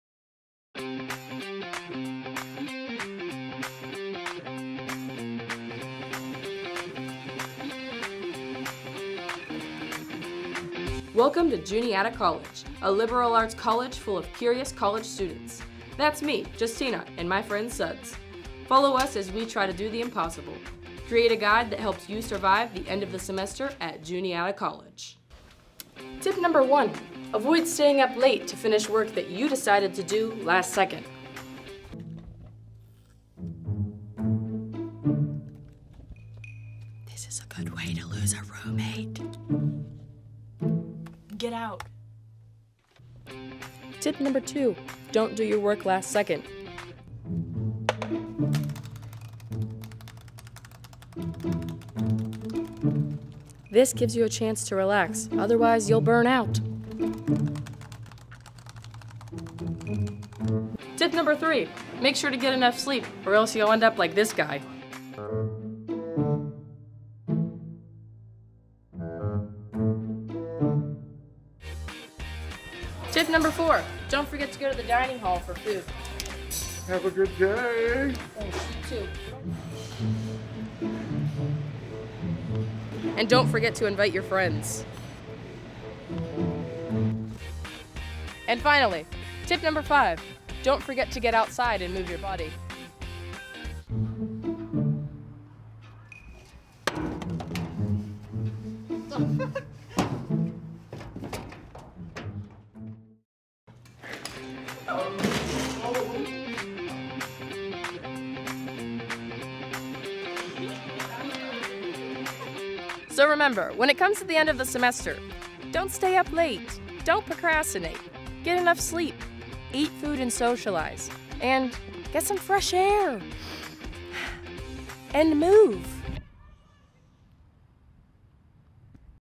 Declassified: End of Semester Survival Guide | Student Life at Juniata College
With finals week quickly approaching, Justinna Brown '23 and Suds Arran '23 walk us through some crucial tips to survive the end of the semester